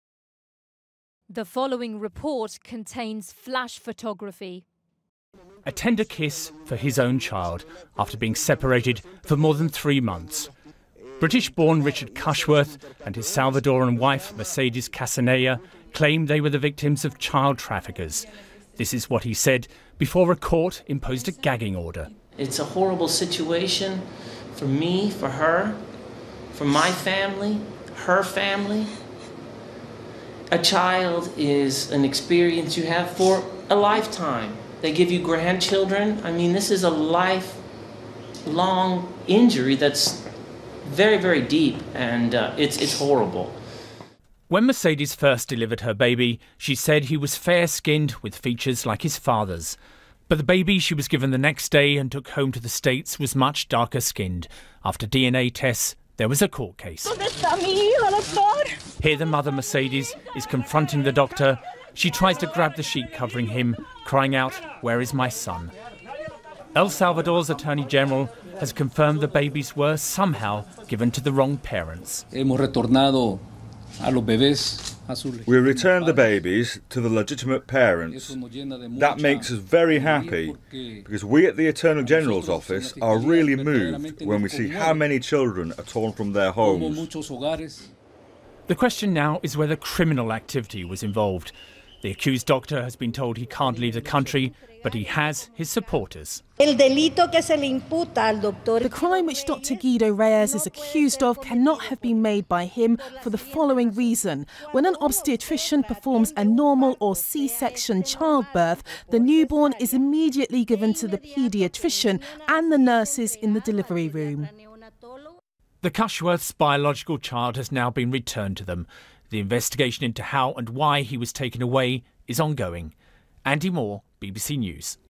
El Salvador baby swap couple reunited with child
A British father and his wife have been reunited with their newborn son after accusing a doctor at a hospital in El Salvador of swapping him.

Richard Cushworth and his Salvadoran wife Mercedes Casanella said they had been victims of child traffickers.

A Salvadorean judge ordered the switch following a DNA test, four months after the baby was born. The other child has been returned to his family.